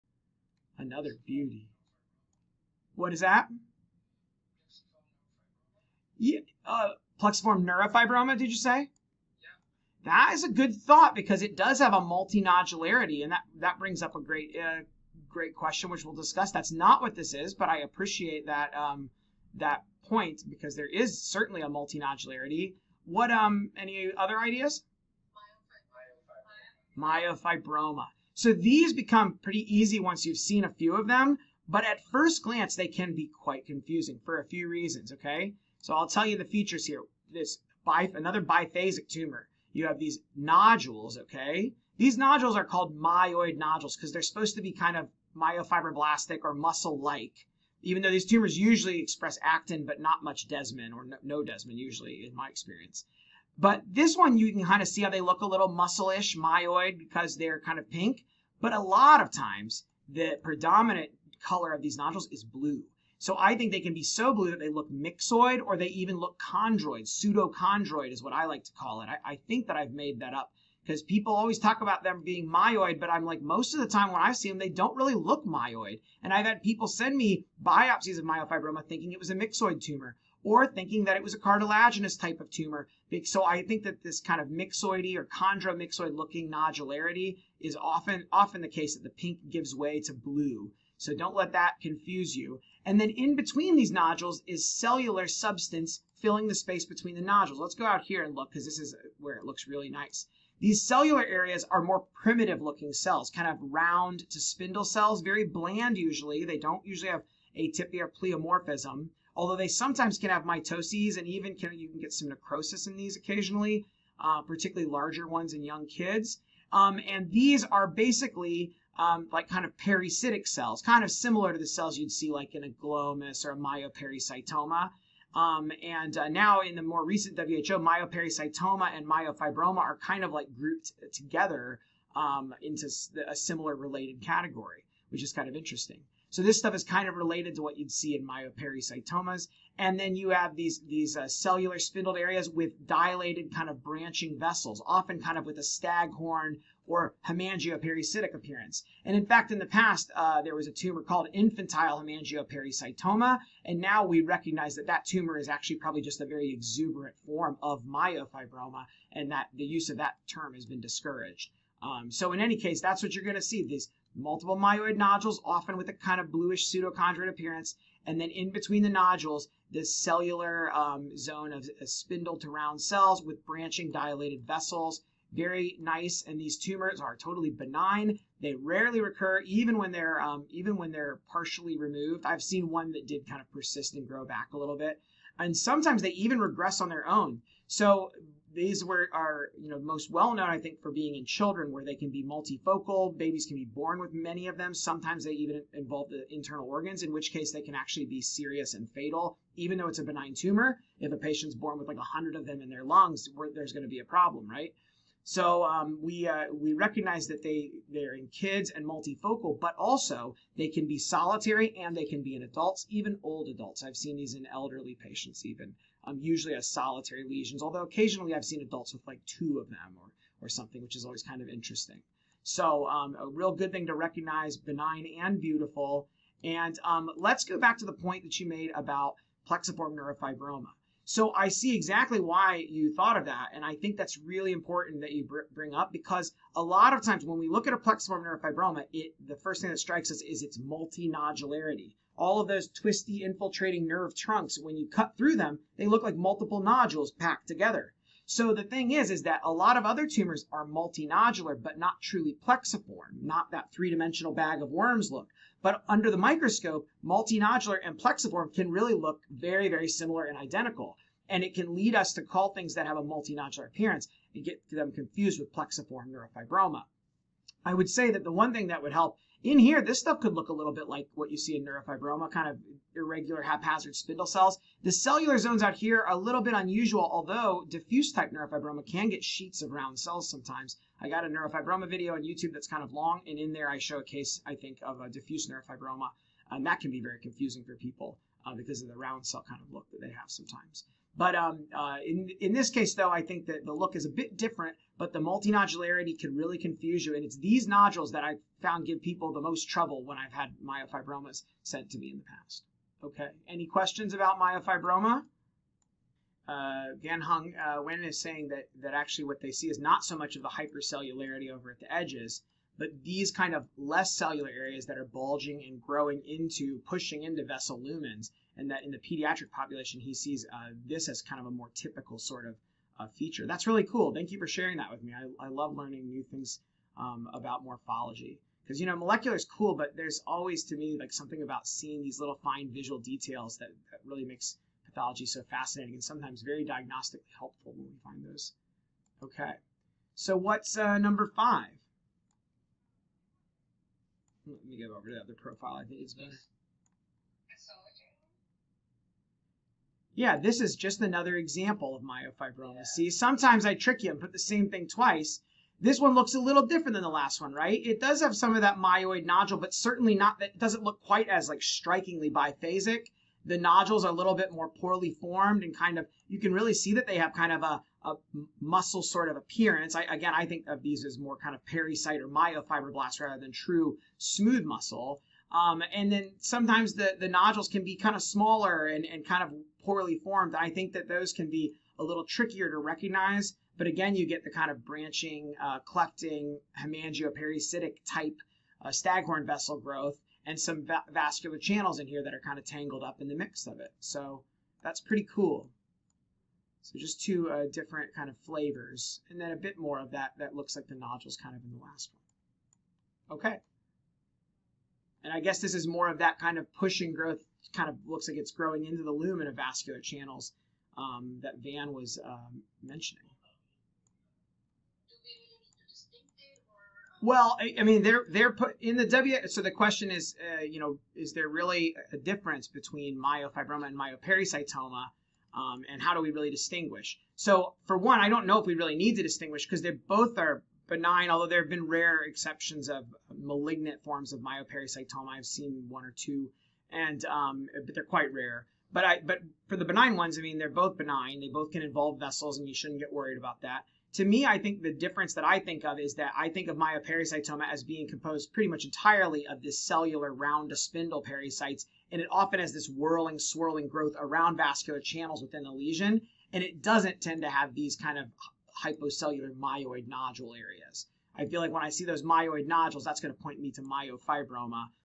Myofibroma & Myofibromatosis 101
Recorded from zoom teaching session for the awesome McGill University pathology residents (and a few faculty too) on September 25, 2020.